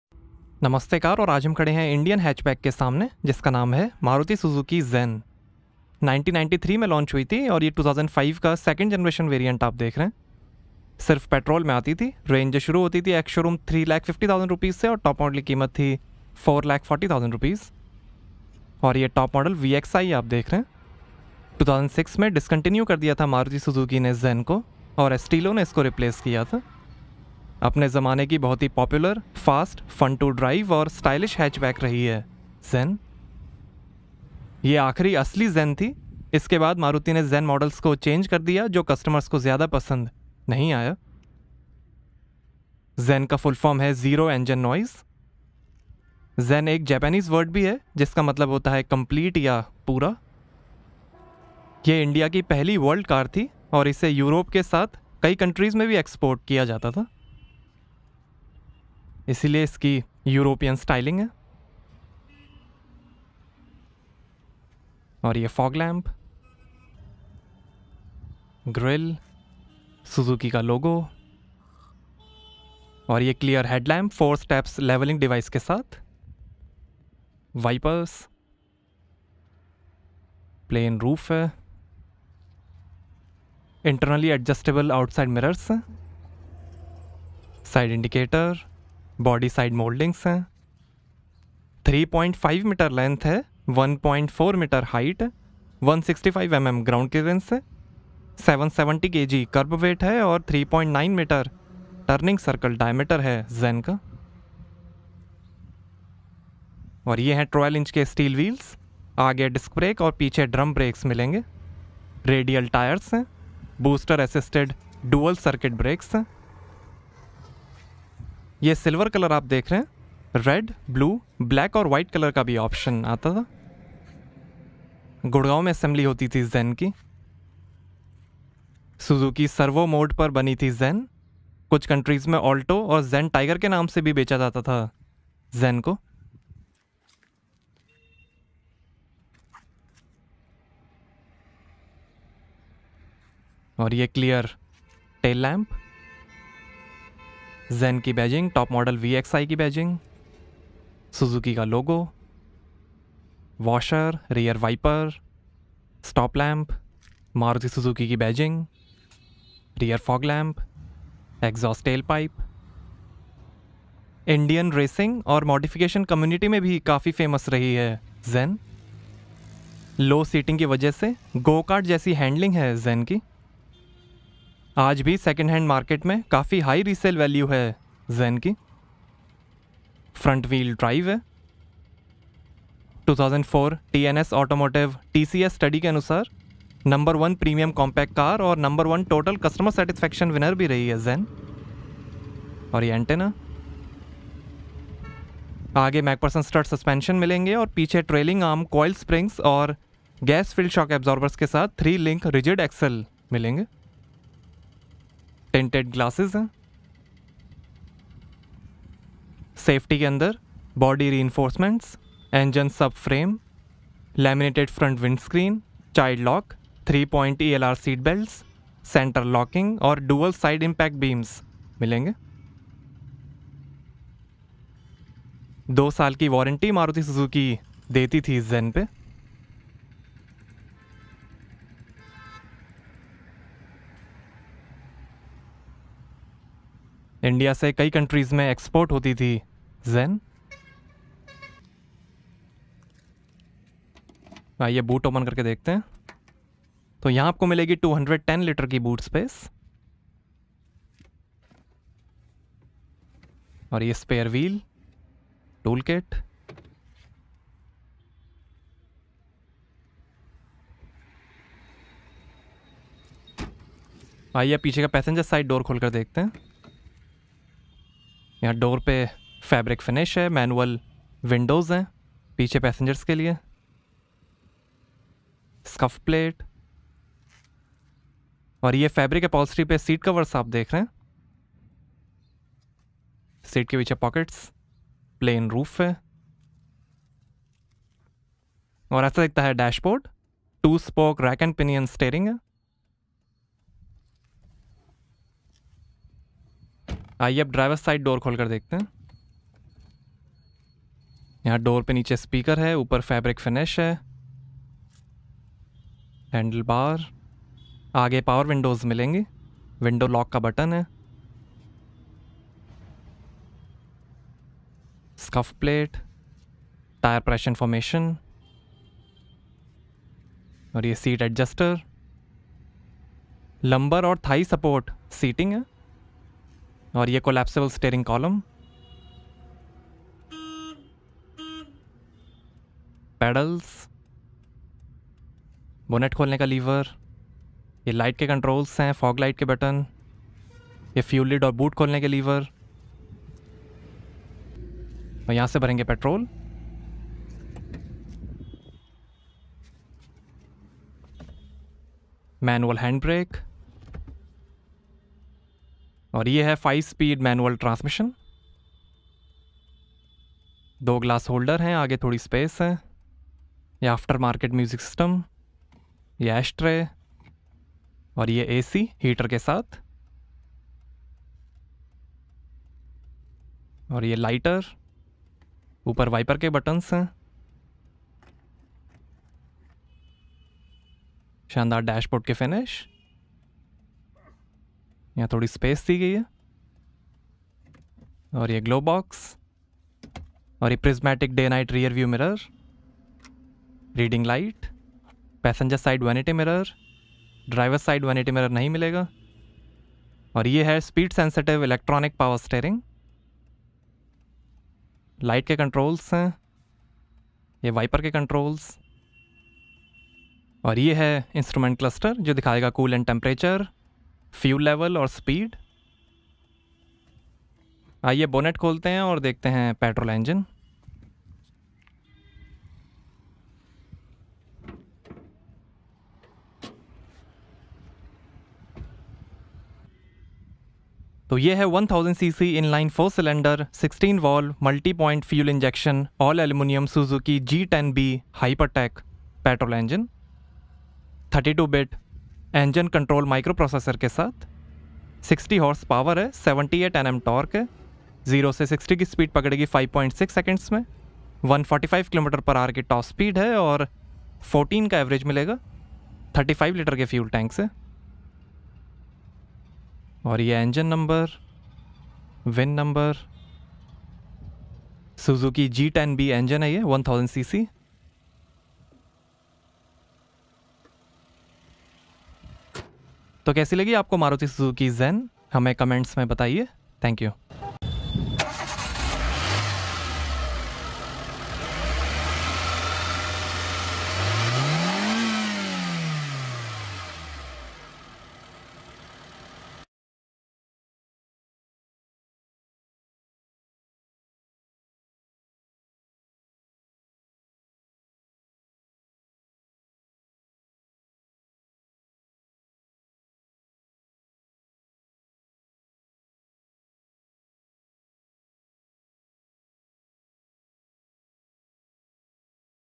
Maruti Suzuki Zen VXi 2005- ₹4.40 lakh | Real-life review
The Maruti Zen is a 5-door hatchback produced and sold in India by Suzuki's Indian subsidiary Maruti Suzuki. It has acquired significant popularity in India since the nameplate was first introduced in 1993. The word "ZEN" is an acronym standing for Zero Engine Noise. It also stands for the Japanese word "Zen" which means 'Complete'. Based on the Suzuki Cervo Mode, the first generation Maruti Zen was introduced on 22 May 1993 (MH410). While it looks very similar to the Cervo Mode, it has a lot of unique sheetmetal as it is ten centimeters longer and ten wider. In Europe and many other export markets, the car sold as the Suzuki Alto.

Petrol: 993 cc, Inline-4 cylinder, 16 Valve, Multipoint fuel injection, All aluminium Suzuki G10B Hypertech engine with 32 bit engine control microprocessor, 60 HP power, 78 Nm torque, 0-60 kmph in 5.6 seconds, 145 kmph top speed, 14 kmpl average, 35 litre fuel tank, 5-speed manual transmission

3 Variants: LX, LXi, VXi

1 Fabric Upholstery

6 Colours: Red, Silver, Metallic Pearl Silver, Blue, Black, White

Price range (Discontinued): Rs. 3.50 lakh - Rs. 4.40 (Ex-showroom, Delhi)